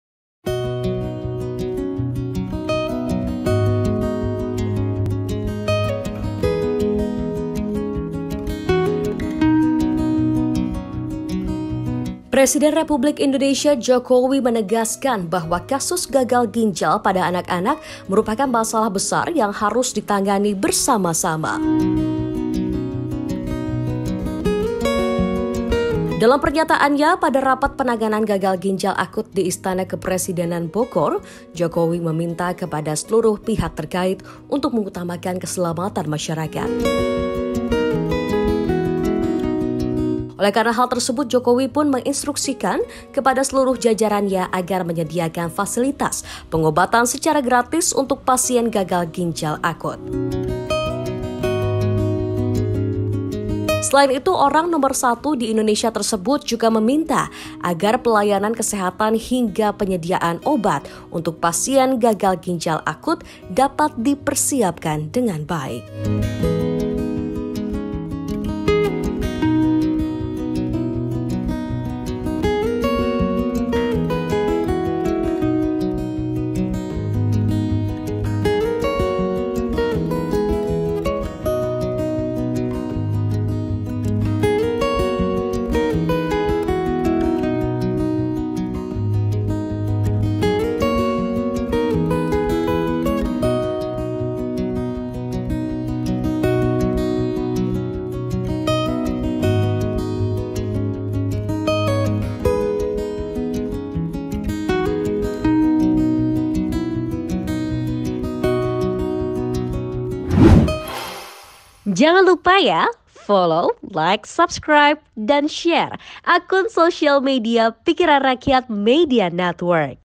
Presiden Jokowi Instruksikan Pengobatan Gratis bagi Pasien Gagal Ginjal Akut
Presiden RI Joko Widodo (Jokowi) menegaskan bahwa kasus gagal ginjal pada anak-anak merupakan masalah besar yang harus ditangani bersama-sama.

Dalam pernyataannya pada rapat Penanganan Gagal Ginjal Akut di Istana Kepresidenan Bogor, Jokowi meminta kepada seluruh pihak terkait untuk mengutamakan keselamatan masyarakat.

"Saya ingin kita semua memberikan perhatian bersama. Yang pertama, utamakan keselamatan masyarakat, jangan menganggap ini masalah kecil. Ini masalah besar," katanya, dikutip pada Selasa, 25 Oktober 2022.

Oleh karena hal tersebut, Jokowi pun menginstruksikan kepada seluruh jajarannya agar menyediakan fasilitas pengobatan secara gratis untuk pasien gagal ginjal akut.

"Saya minta diberikan pengobatan gratis kepada pasien-pasien yang dirawat. Saya kira ini penting sekali," ujarnya.

Selain itu, orang nomor satu di Indonesia tersebut juga meminta agar pelayanan kesehatan hingga penyediaan obat untuk pasien gagal ginjal akut dapat dipersiapkan dengan baik.